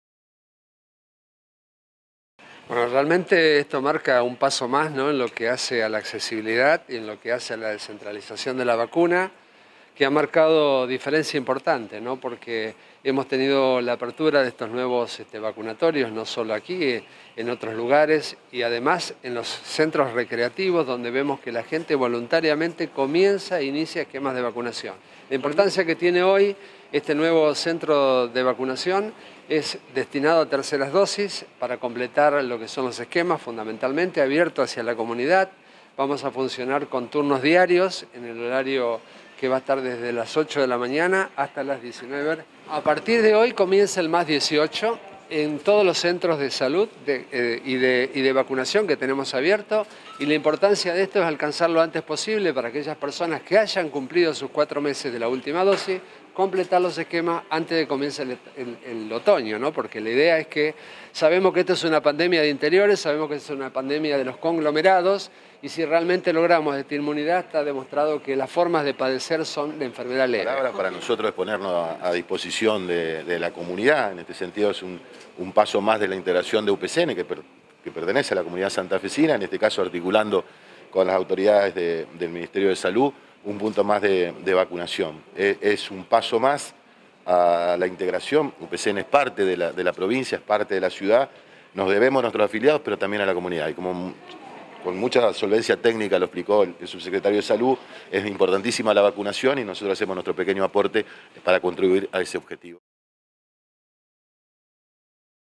COVID-19: LA PROVINCIA LANZÓ LA VACUNACIÓN LIBRE DE TERCERAS DOSIS PARA MAYORES DE 18 AÑOS
Las mismas se aplicarán a los cuatro meses de la segunda dosis.